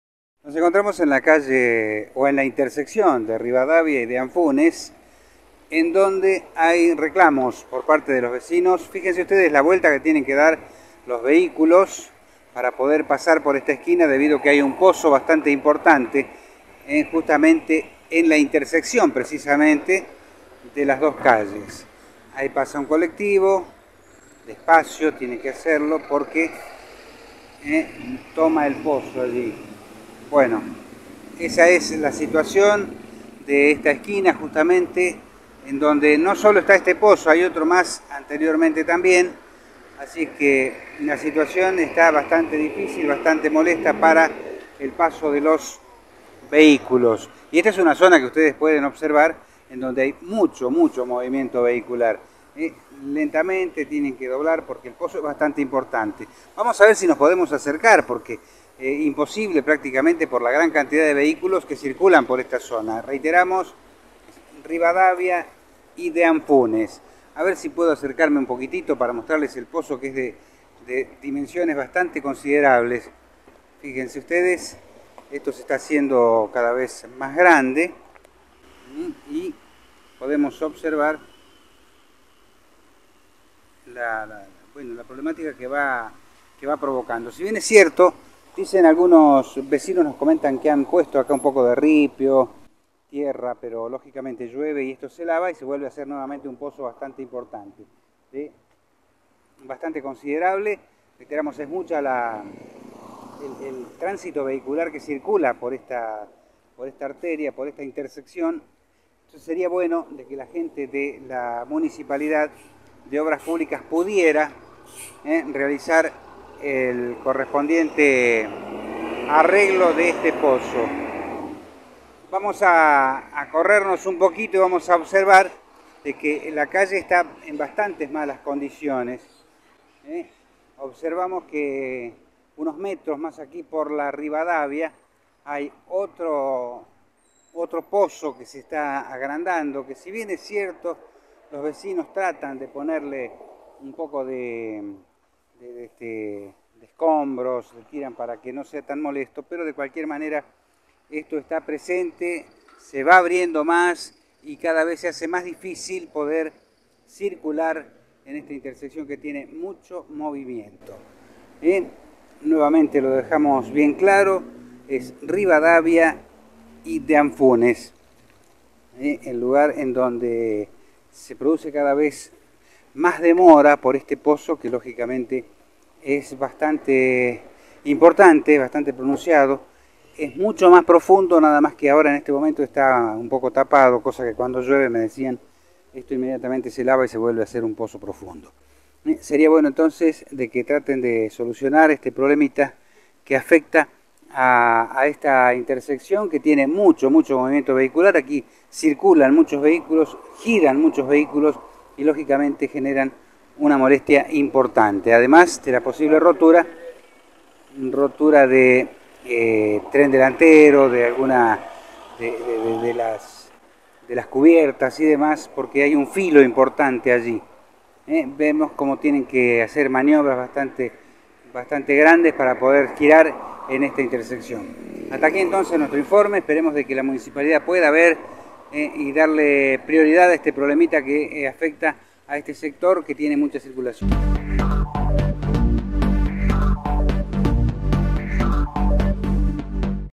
SALTA - Esquina Peligrosa: Pozo de gran tamaño entorpece el tránsito
Rivadavia y Deán Funes